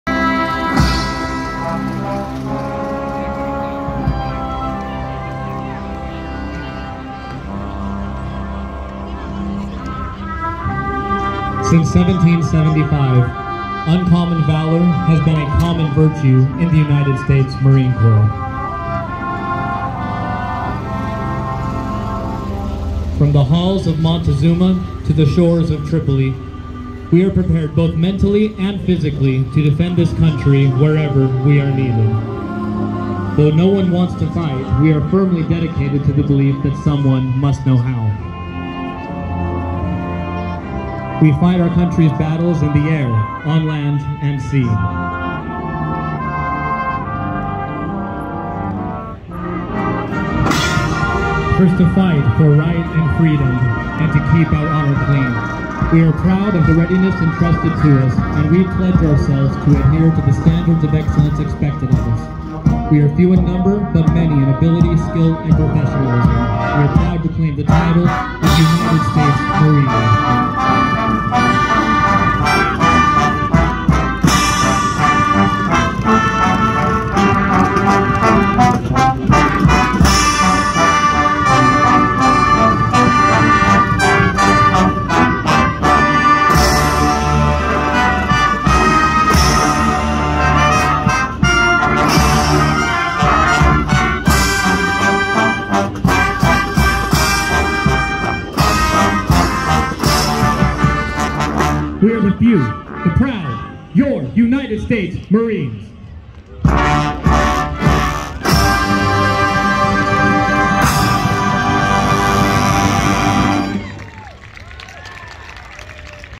Marching Bronco Xpress of Fayetteville State University @ UNC-Pembroke: Friday, September 1, 2023
Military Day: Mass Band Half-Time Performance by The MBX, UNC-Pembroke, and the U.S. Marine Corps